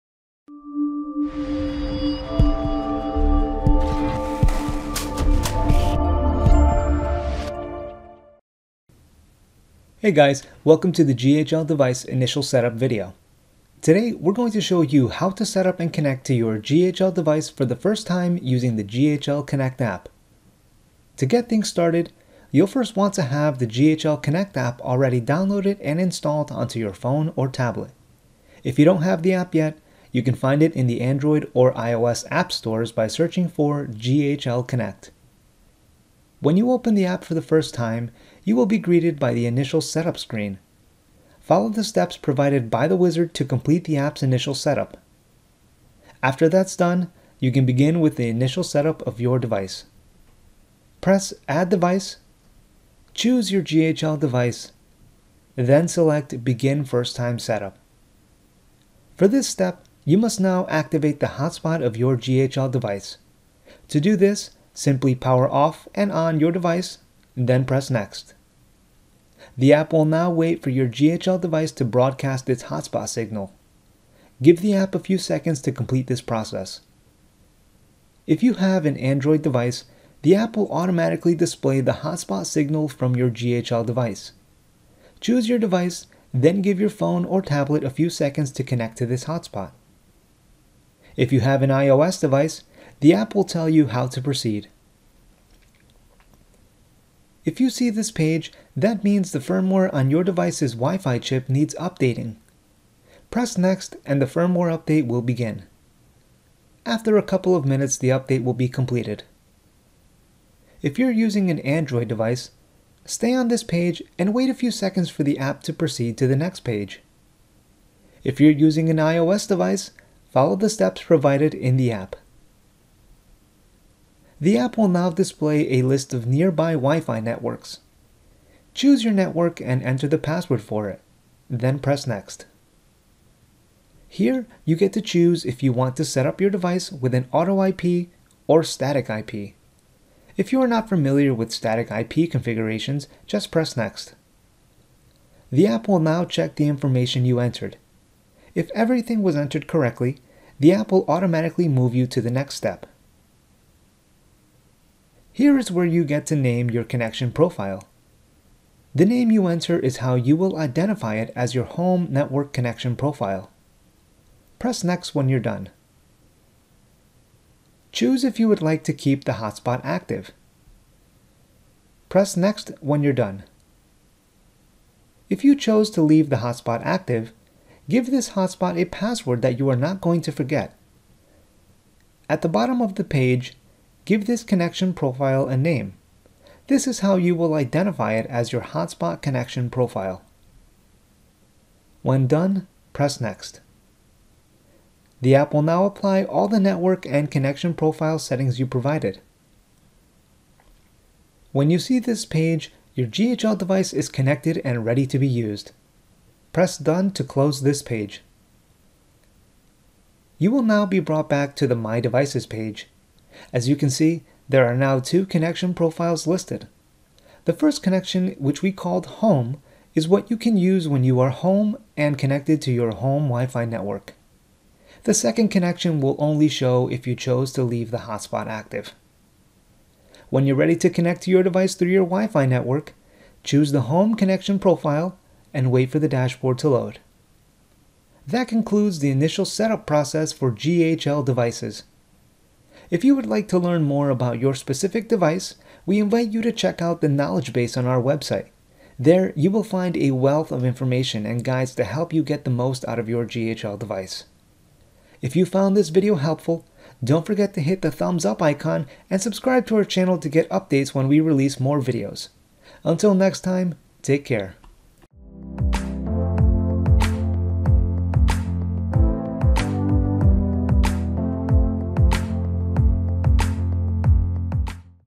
GHL Device Initial Setup (How-to)
From beginning to end, this video will show you how to get your GHL device connected to your Wi-Fi network in less than 5 minutes.

Video applies to the following products:
* ProfiLux 4 / 4e / IL
* ProfiLux Mini Wi-Fi / Light Wi-Fi
* Doser 2.2 Standalone / 2.2 Maxi Standalone / IL
* Mitras LX 7 LED

For more information on setting up your device, we highly recommend visiting our Knowledge Base. Start with the link below to get you started with the basics.